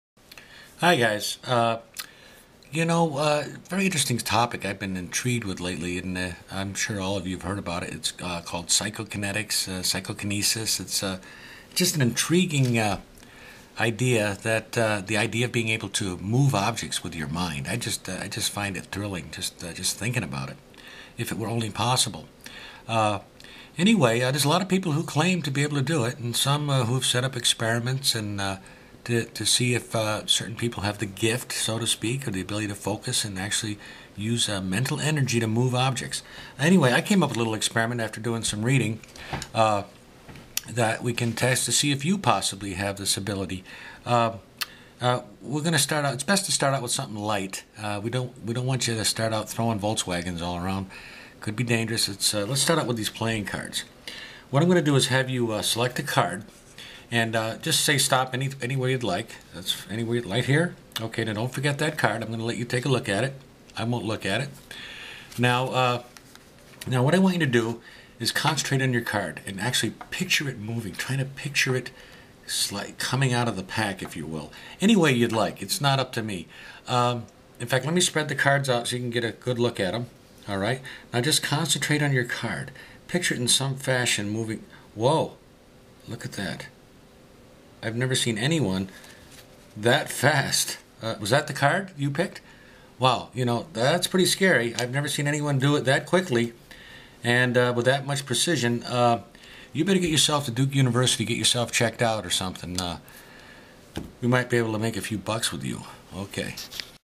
The Open and Eerie Spinout
This is my version of Earl Nelson's and Ken Krenzel's Ghost card/Eerie Spinout type effect. Totally impromptu, and you don't have to put any work in the Spectator's card ala Ken's method.